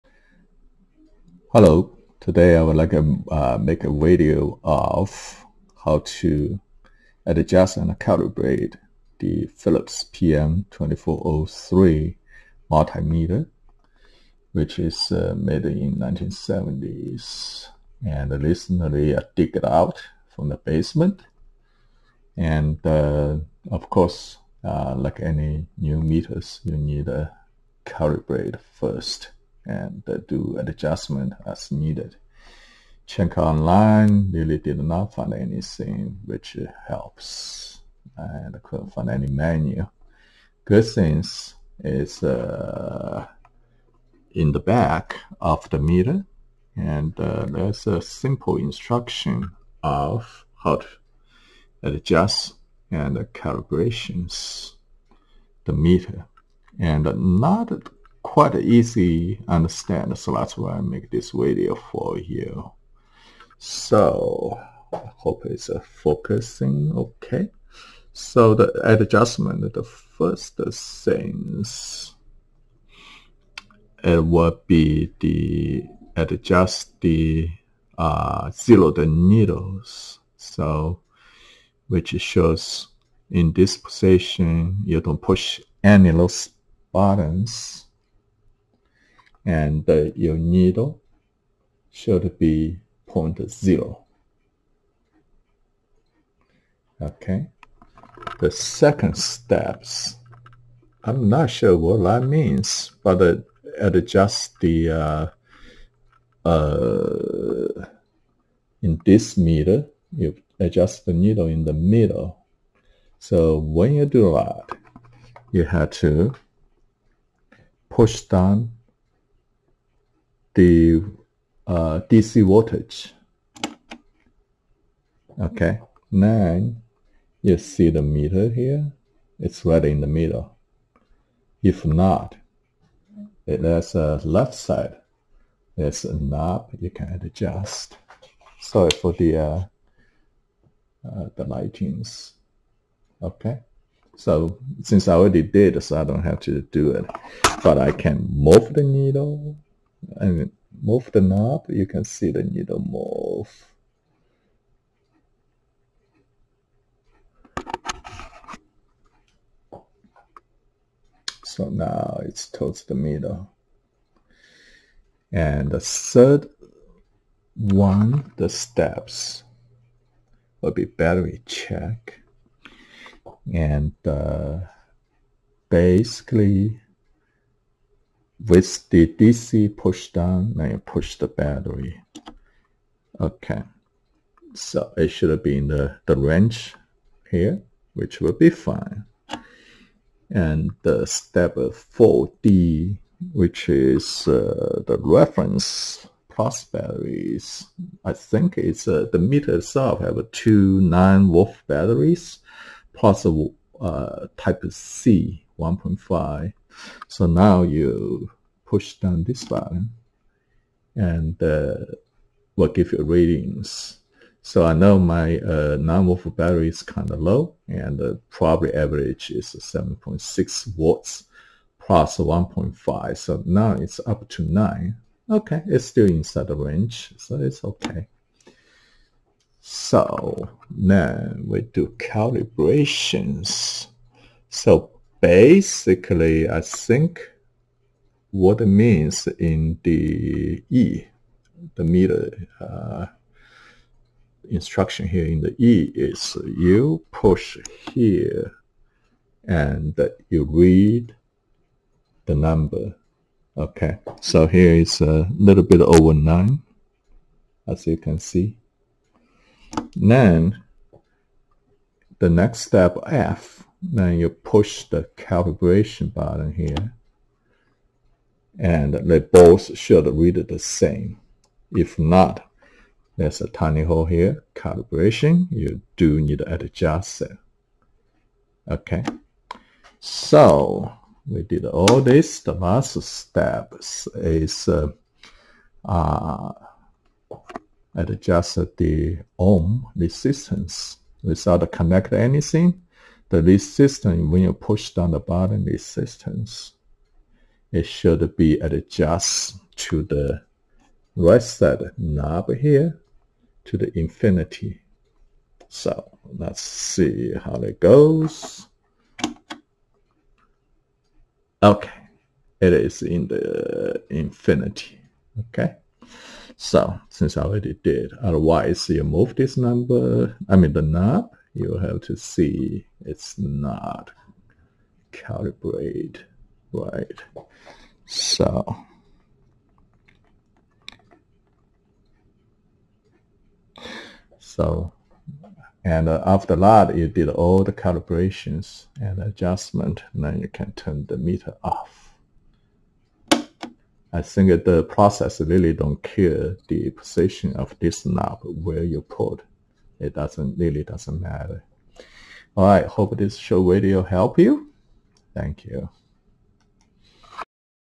Vintage Philips PM 2403 analog multimeter adjustment and calibration
Unable to find the information online so I decided to make this short video to share .

Recently retired and decided to pick up the electronics as a hobby, dig out  this old  analog meter for the bench with my other Fluke DMMs.